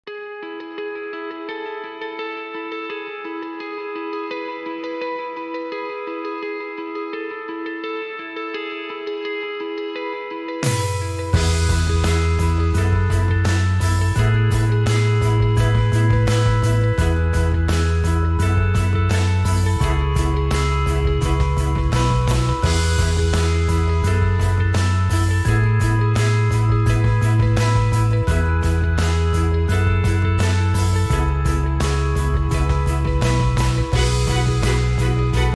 Close-Sesame™ Double-Sided Tape
In the fast-paced world of e-commerce, packaging is key to meeting consumer expectations and operational efficiency. H.B. Fuller leads this transformation with our innovative Close-Sesame™ double-sided tape.